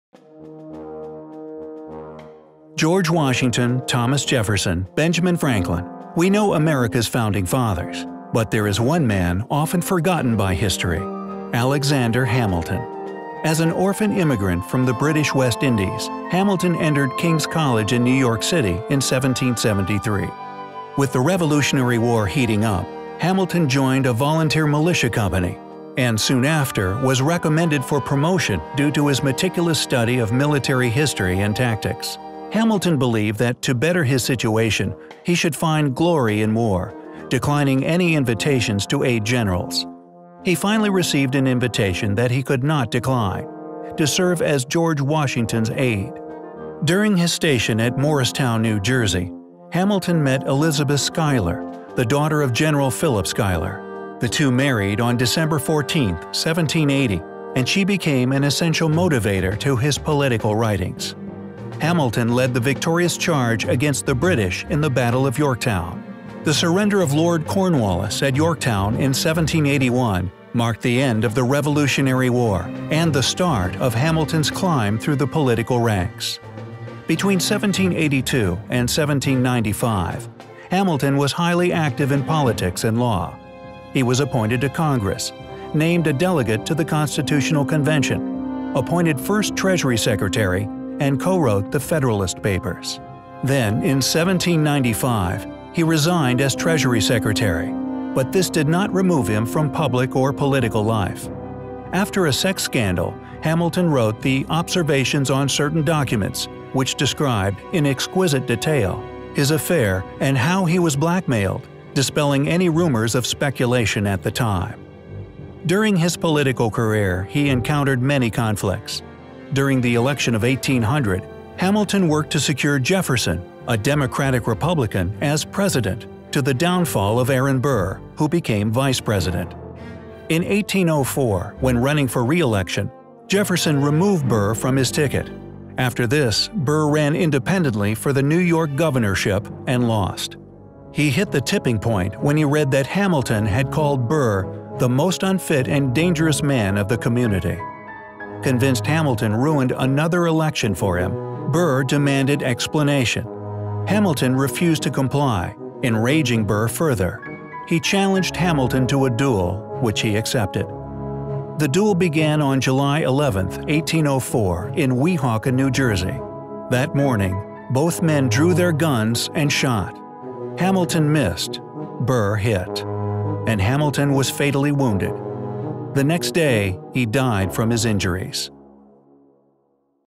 The Ten Dollar Founding Father
A short documentary piece on the life and death of one of the most important Founding Fathers of the United States of America: Alexander Hamilton.